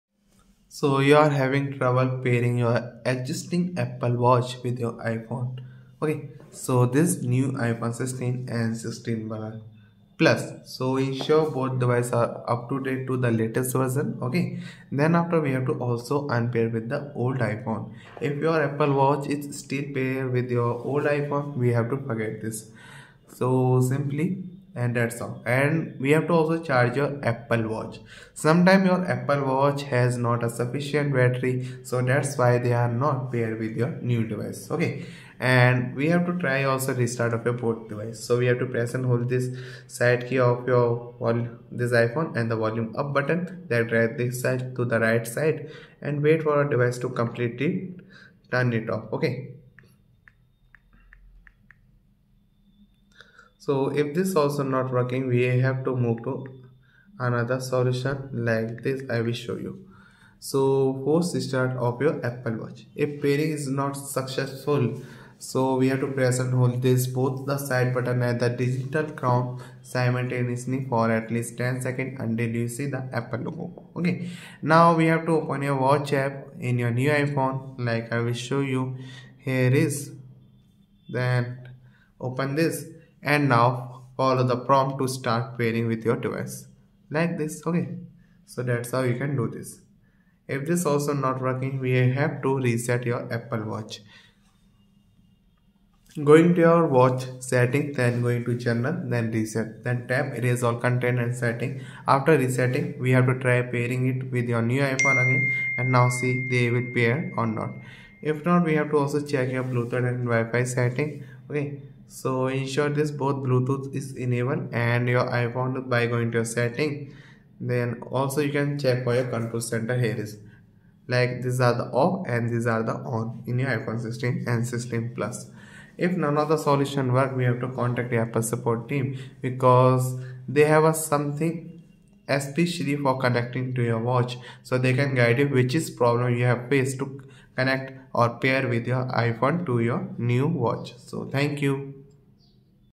Existing Apple Watch Not Pairing With New Iphone 16/16 Plus: Here’s How To Fix
In this informative video, we address the common issue of pairing an existing Apple Watch with the new iPhone 16 and iPhone 16 Plus. If you are experiencing difficulties in establishing a connection between your devices, this guide will provide you with step-by-step solutions to resolve the problem.

00:17 Fix 1: Forget Old Device
00:36 Fix 2: Restart Both Device
01:03 FIX 3: FORCE RESTART
01:33 Fix: 4 Reset Watch